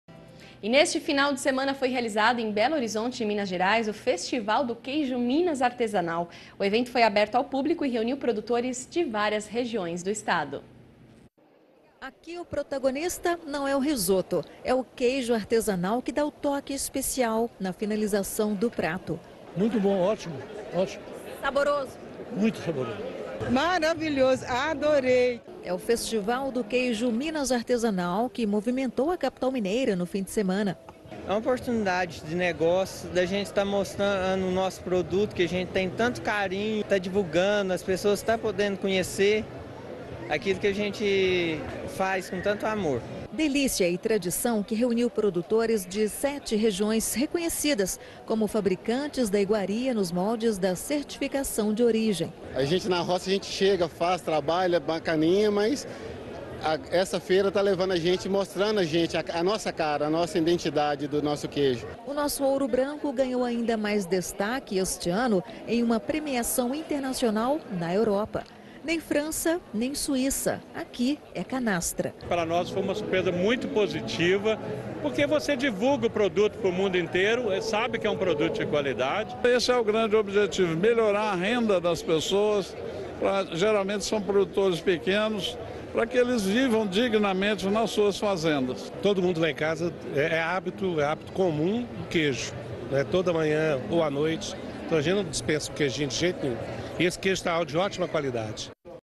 Festival do Queijo reúne produtores em Belo Horizonte
E neste final de semana foi realizado em Belo Horizonte, Minas Gerais, o Festival do Queijo Minas Artesanal. O evento foi aberto ao público e reuniu produtores de várias regiões do estado. O Festival do Queijo Minas Artesanal continuou neste domingo (28), na serraria Souza Pinto, em Belo Horizonte.

O evento reuniu produtores das regiões de Araxá, Campo das Vertentes, Canastra, Cerrado, Serra do Salitre, Serro e Triângulo e contou com oficinas de harmonização, maturação e conservação do produto. Assim como no sábado (27), neste domingo aconteceu a oficina de harmonização do queijo minas artesanal com cervejas artesanais mineiras.

Confira os detalhes!